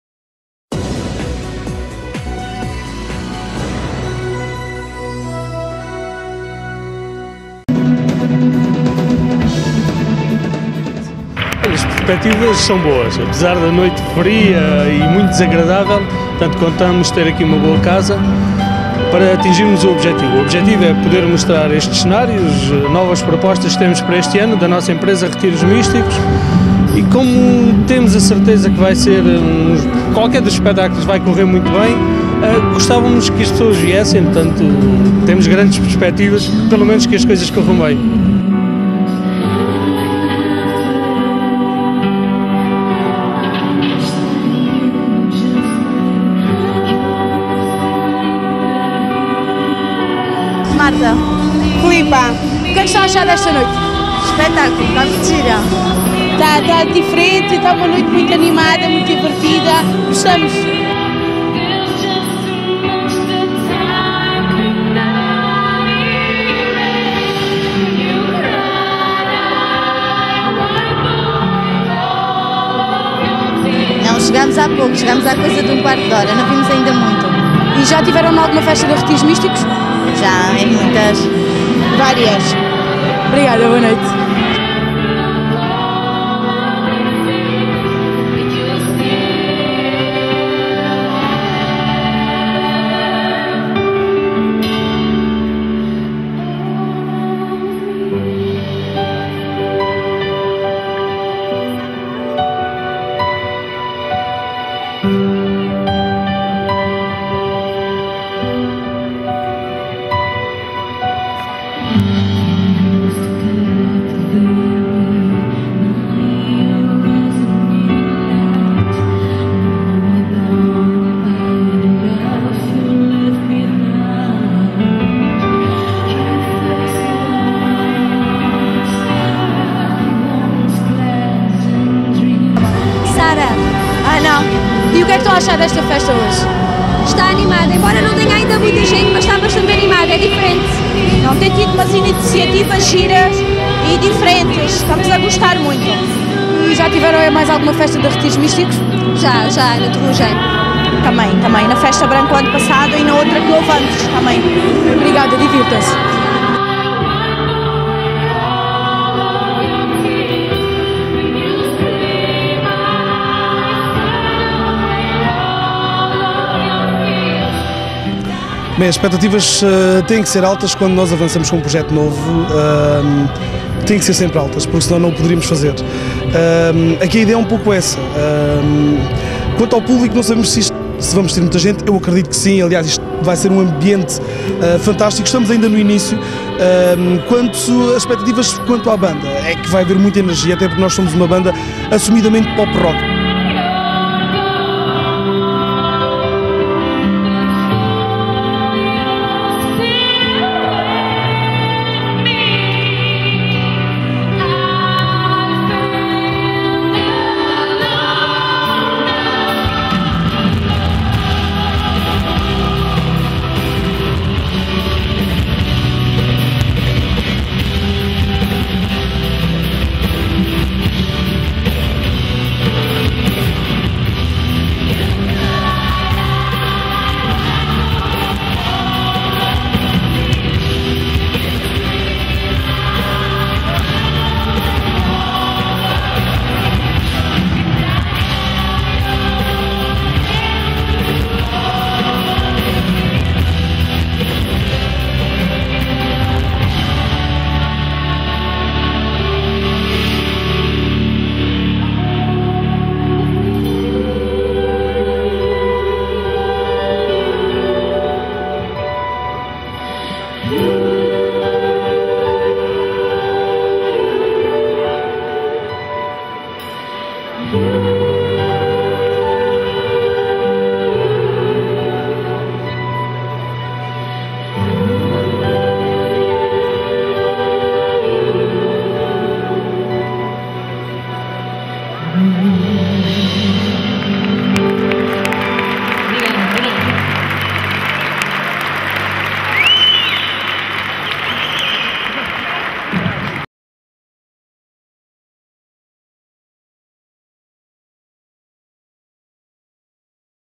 "Mystic Projects Tour 2012" - Coliseu Elvas
Cerca de 1 milhar de pessoas terão estado presentes este sábado no Coliseu José Rondão Almeida em Elvas, para assistirem ao espetáculo "Mystic Projects Tour 2012".